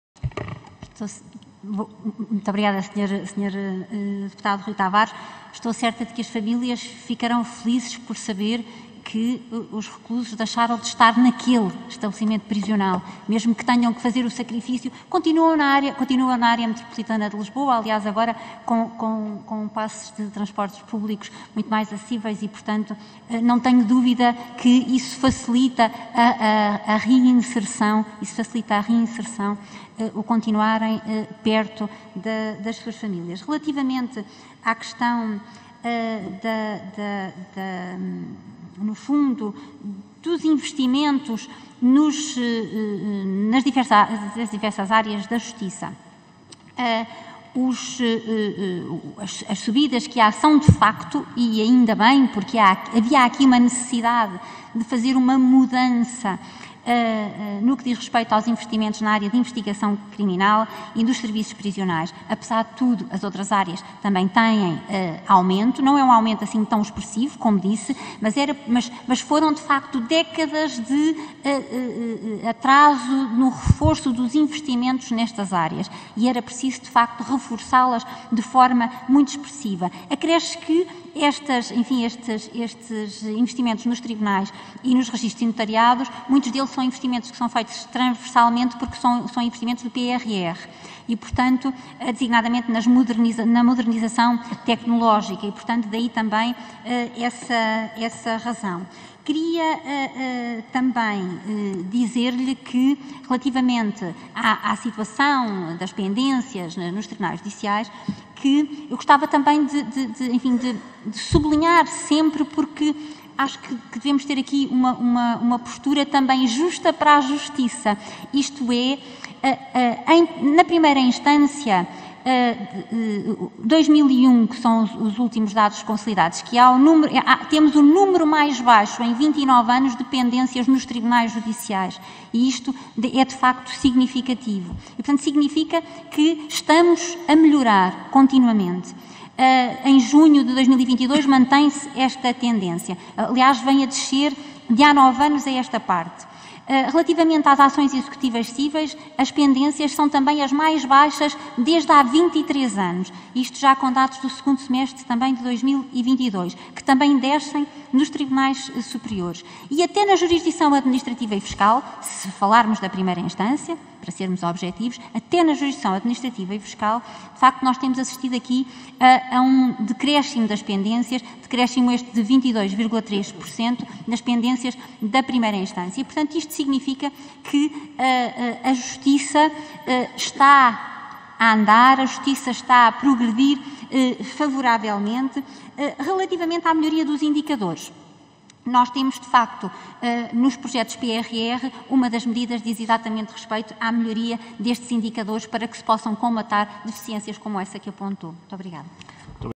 07-11-2022 | OE2023 - Resposta da Ministra da Justiça ao deputado Rui Tavares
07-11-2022 | Resposta da Ministra da Justiça, Catarina Sarmento e Castro, ao deputado Rui Tavares (L), na apreciação na especialidade das Propostas de Lei (GOV) Grandes Opções para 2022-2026 e Orçamento do Estado para 2023 - Comissão de Orçamento e Finanças - Comissão de Assuntos Constitucionais, Direitos, Liberdades e Garantias